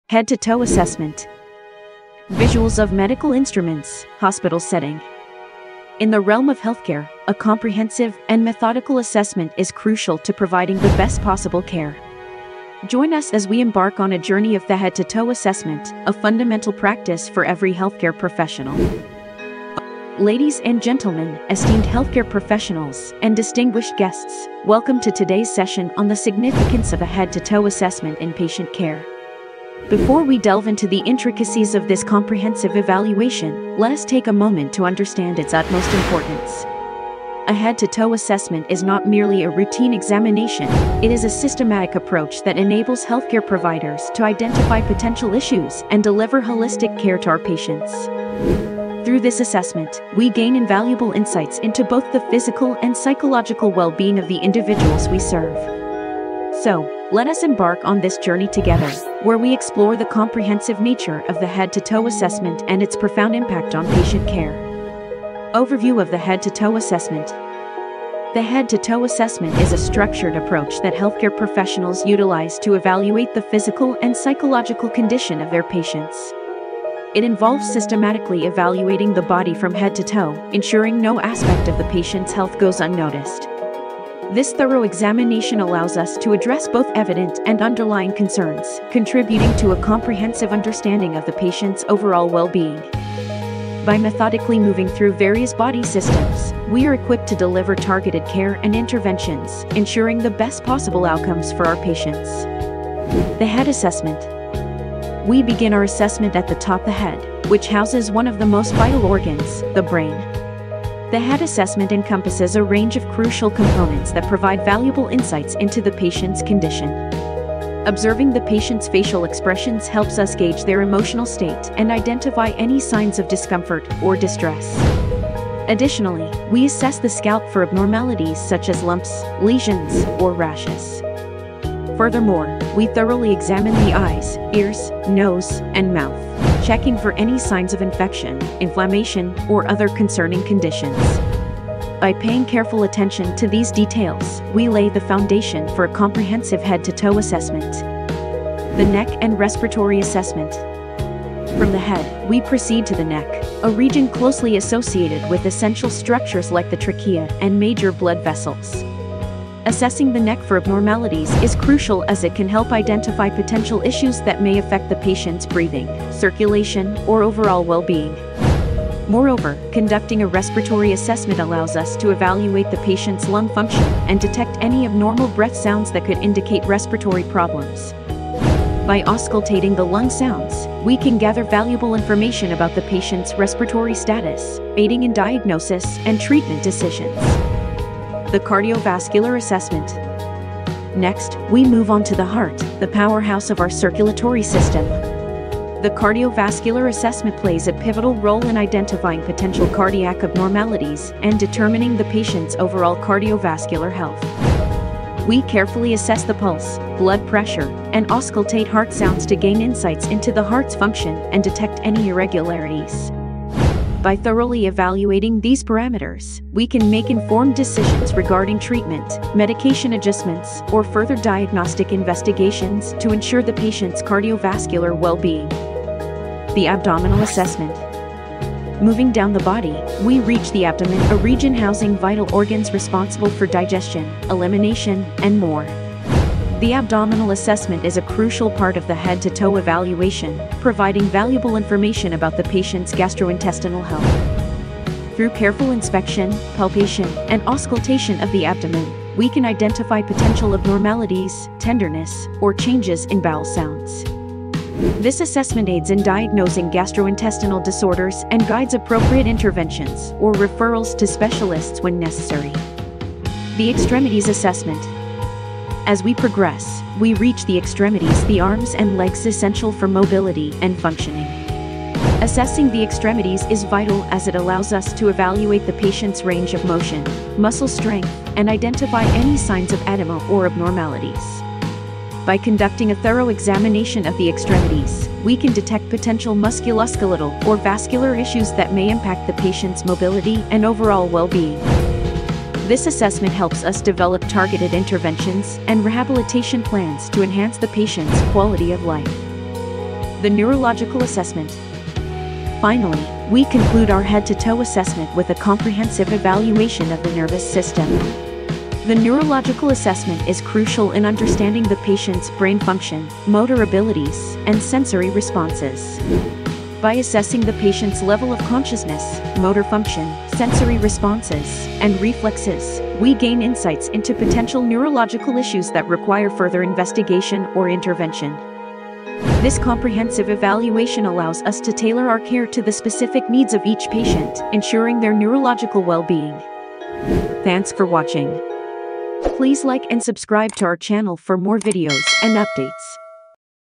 Head to Toe Assessment | nursing
Hello Everyone!
 Welcome to our comprehensive Head to Toe Assessment video, designed specifically for nursing professionals and students. In this detailed tutorial, we cover the essential steps and techniques involved in conducting a thorough nursing assessment from head to toe. Follow along as we demonstrate the proper procedures for assessing vital signs, neurological functions, respiratory system, cardiovascular system, gastrointestinal system, musculoskeletal system, and more. Whether you're a seasoned nurse looking to refresh your skills or a nursing student seeking guidance, this video is a valuable resource for honing your assessment abilities. Watch now and enhance your expertise in performing a comprehensive head to toe assessment in the nursing field.